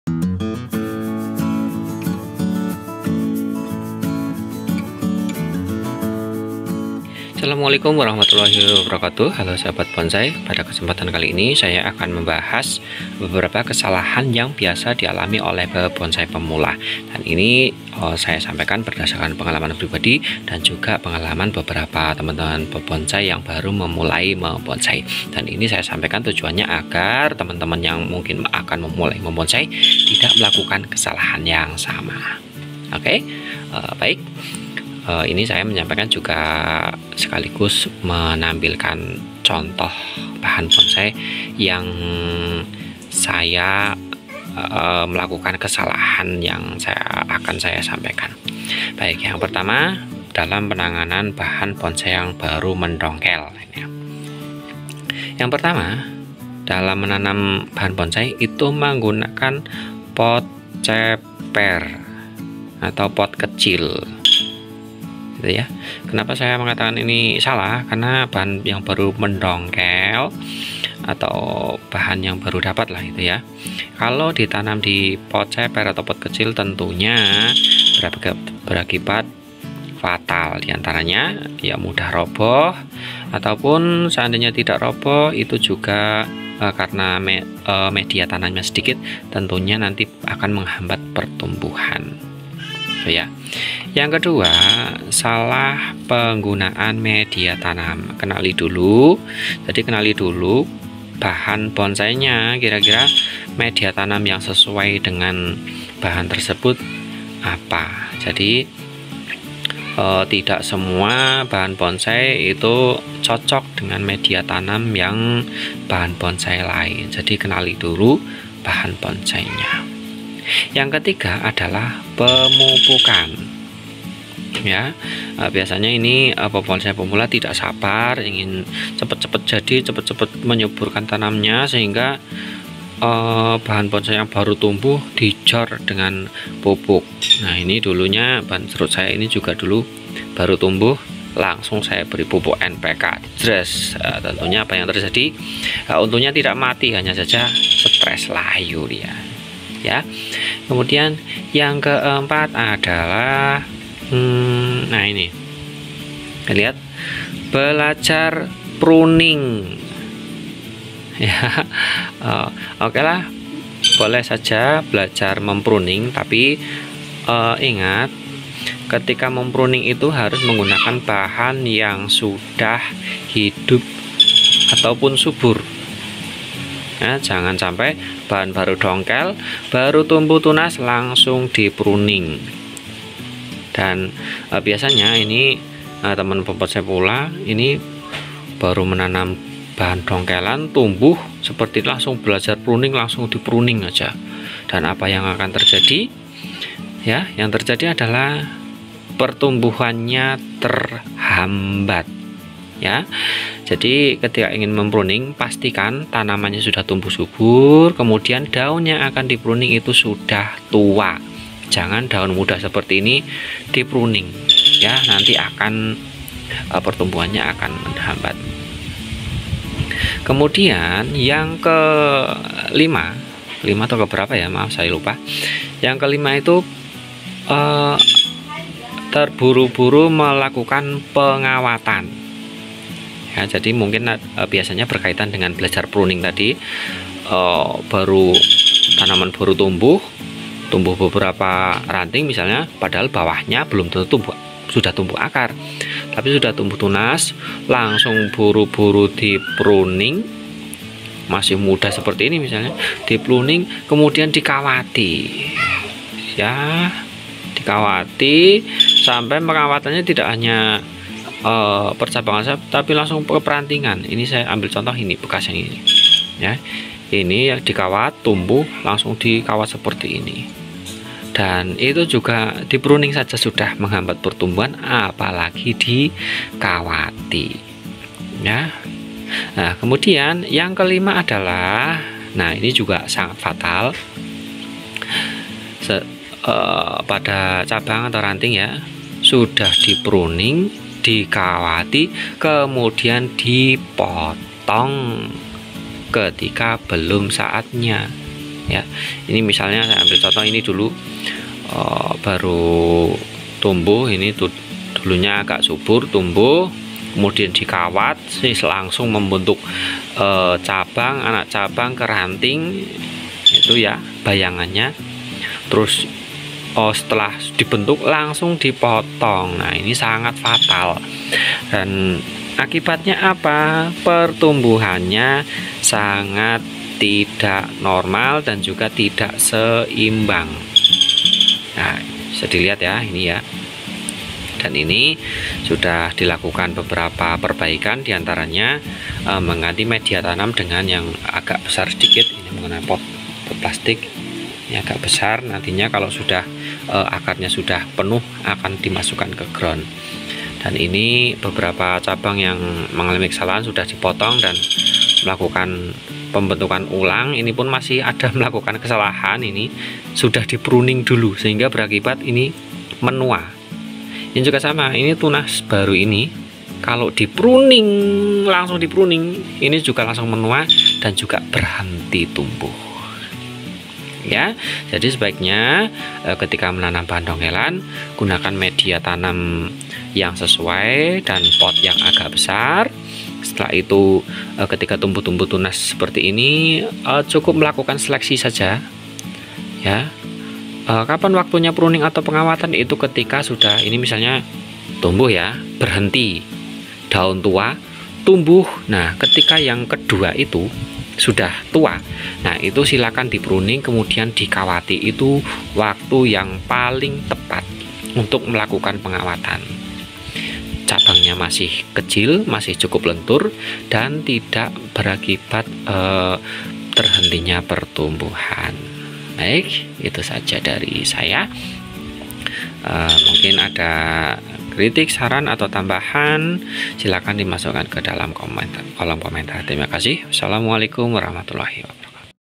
KESALAHAN PEMULA DALAM MEMBUAT BONSAI SERUT ( STREBLUS ASPER ) DARI DONGKELAN
Kesalahan bonsai pemula dalam membuat bonsai ketika memulai membuat bonsai sering kali dialami. Dalam video ini saya menceritakan pengalaman probadi dan juga pengalaman dari teman pebonsai lain yang pernah mengalami kesalahan yang sama khususnya dalam membuat bonsai serut / streblus asper dari dongkelan. Dalam hal ini saya mengartikan juga kasalahan paling umum dalam membuat bonsai. Sering kali kesalahan-kesalahan ini terlihat sederhana tetapi berakibat fatal pada bonsai kita. Jangan sampai bahan bonsai yang bagus menjadi hancur. Saya berharap teman-teman pemula yang akan memulai membonsai tidak melakukan kesalahan yang sama.